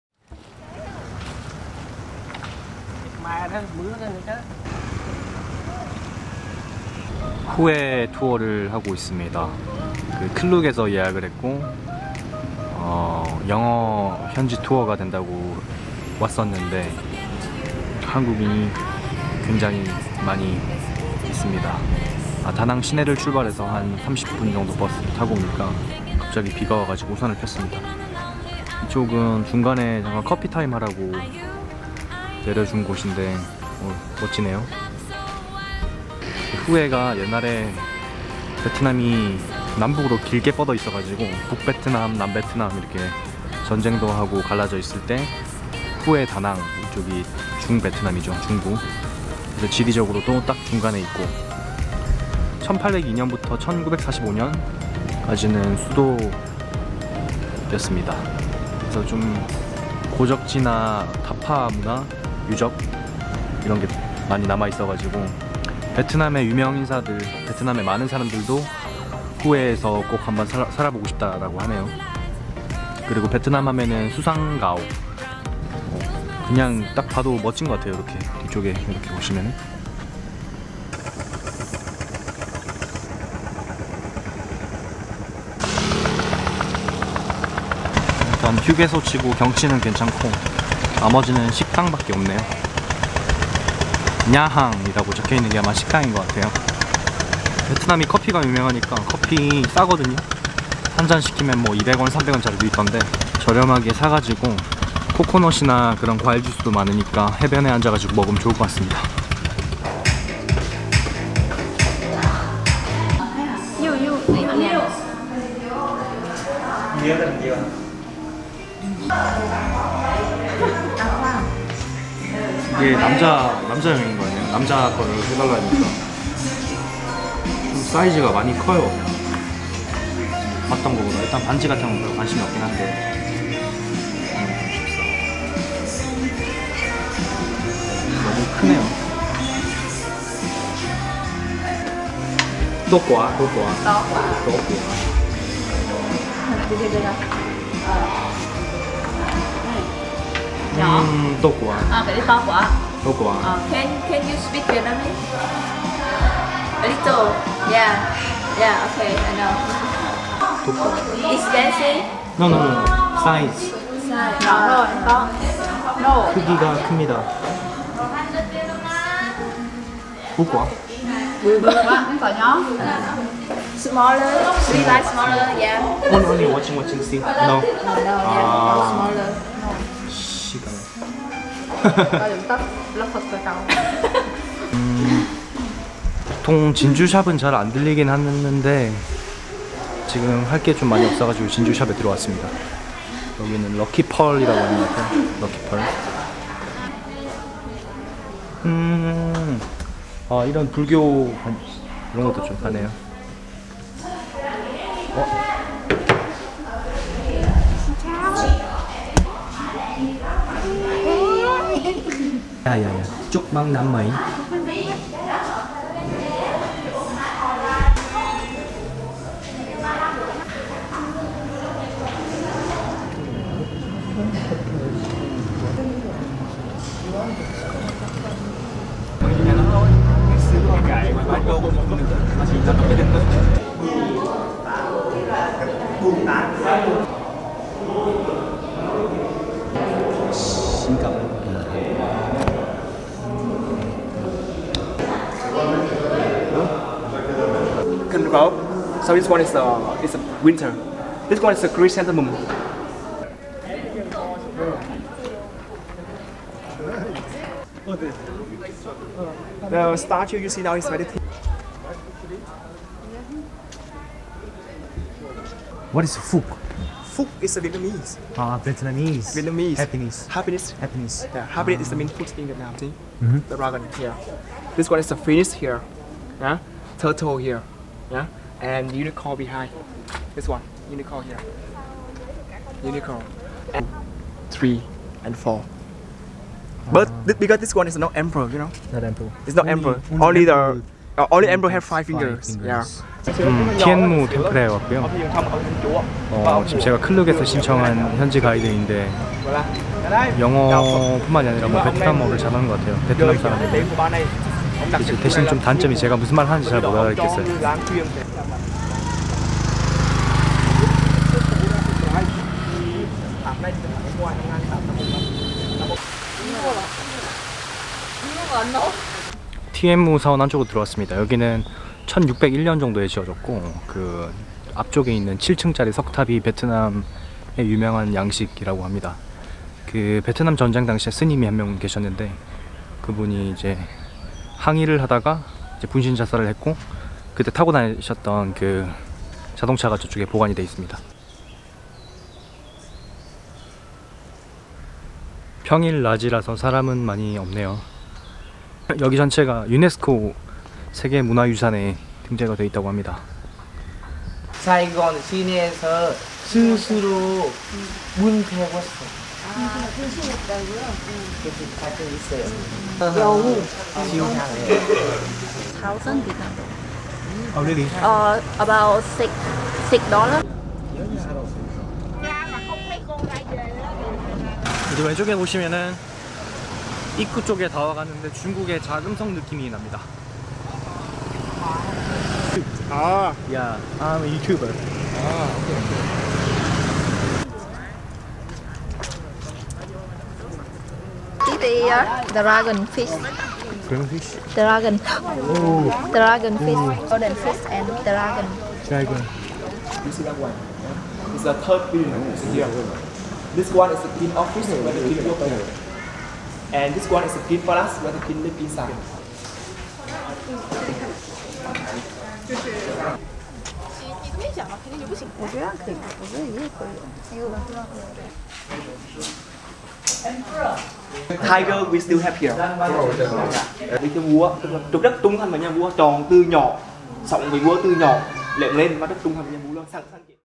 Klook 투어 [베트남 후에 편] / Klook tour Hue, Vietnam
클룩을 이용해서 베트남 다낭 후에 1일 투어를 해보았는데요
예상 가능한 정도의 투어였던것같아요

큰 기대보다는 소소한 재미를 위한다면 추천~